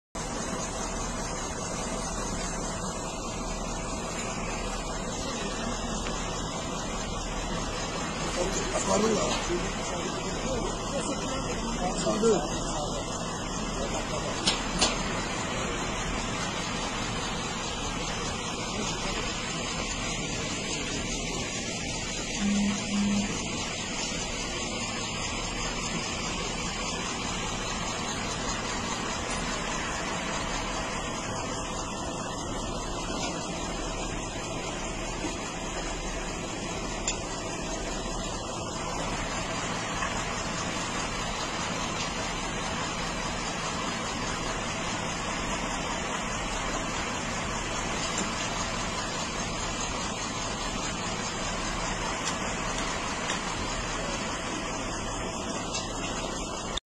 Ռուս խաղաղապահներն ապամոնտաժում են Լաչինի միջանցքում իրենց տեղադրած վրանները
Ադրբեջանական լրատվամիջոցները հայտնում են, որ ռուս խաղաղապահներն ապամոնտաժում են Լաչինի միջանցքում իրենց տեղադրած վրանները։ Այլ մանրամասներ չեն հաղորդվում: